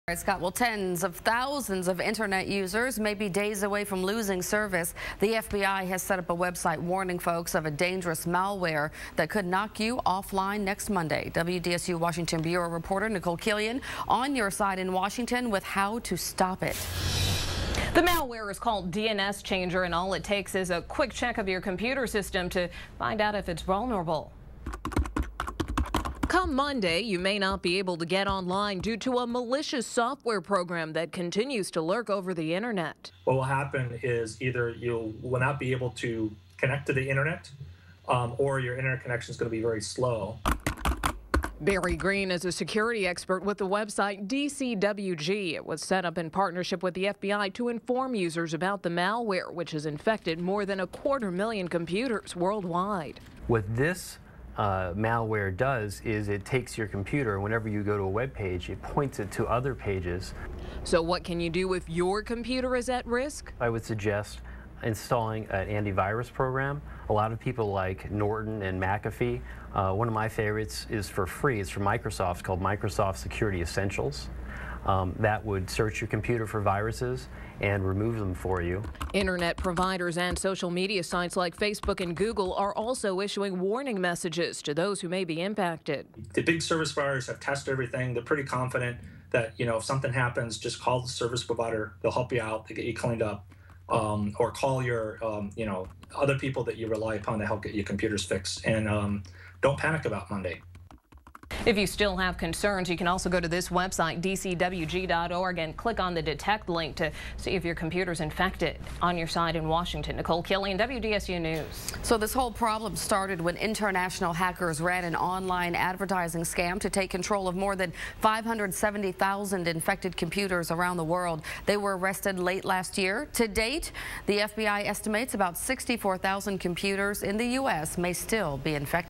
DNS Changer Malware
Brief news on DNS Changer Malware. The FBI is set to turn off their DNS servers on Monday July 9th. This will knock about 70,000 people in the US off the Internet who had been infected with that malware.